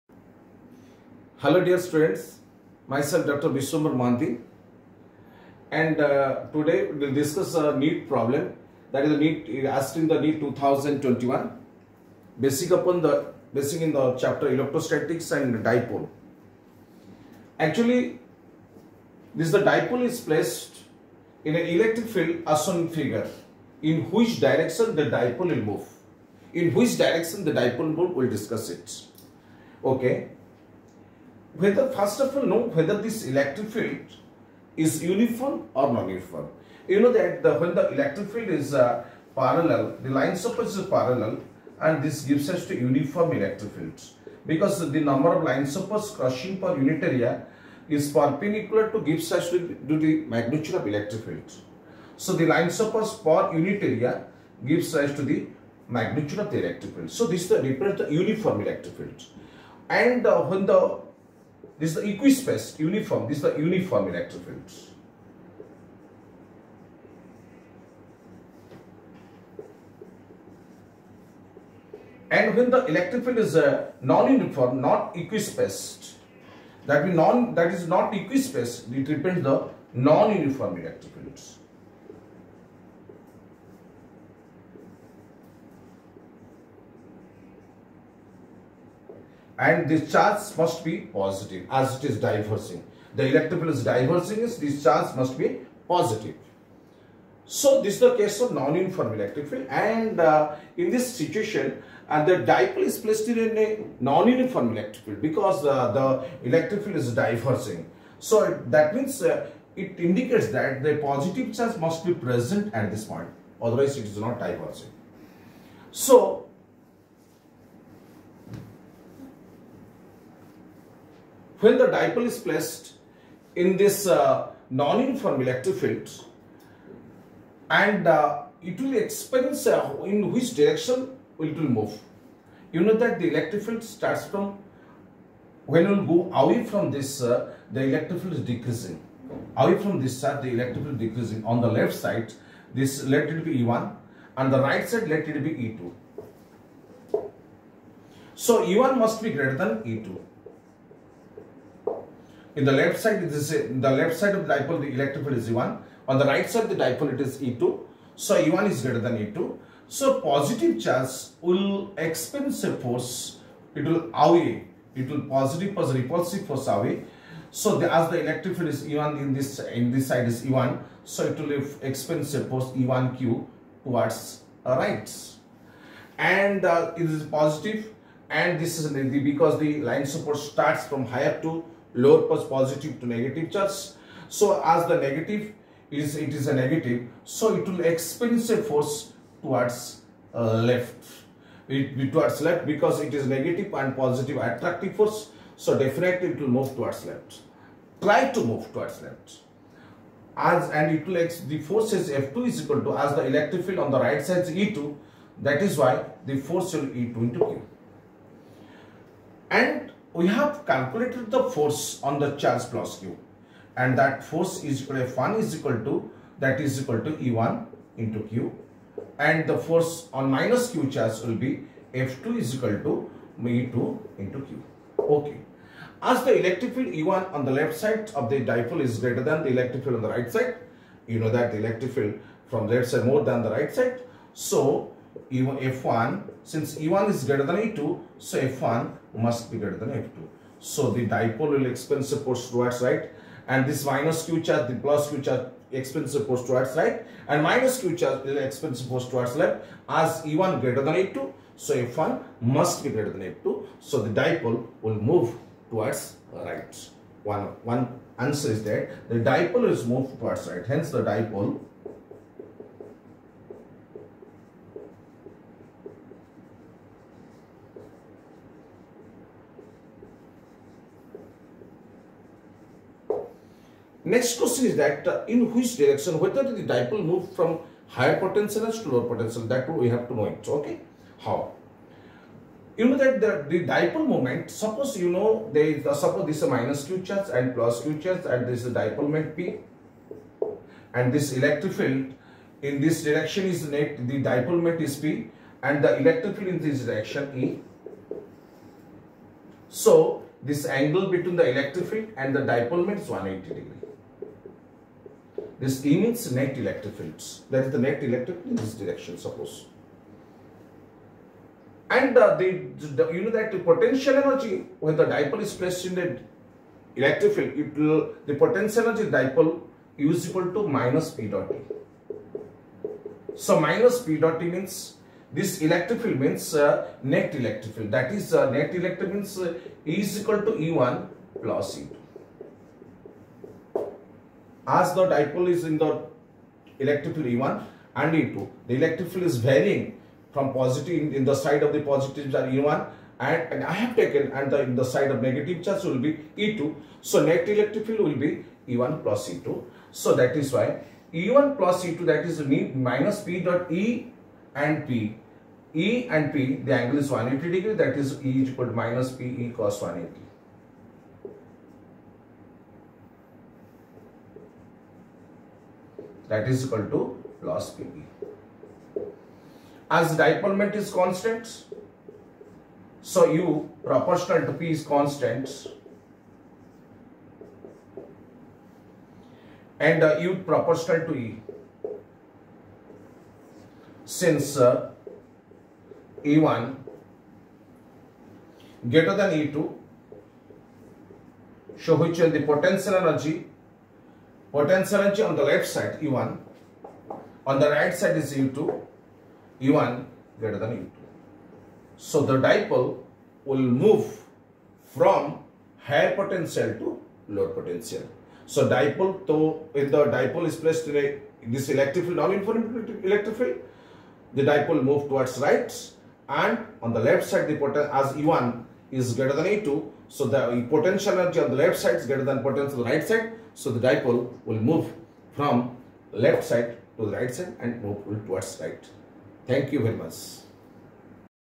ELECTROSTATICS | DIPOLE | NEET 2021 QUESTION | By Dr. BM
Hello students, go through this detailed explanation of NEET 2021 question.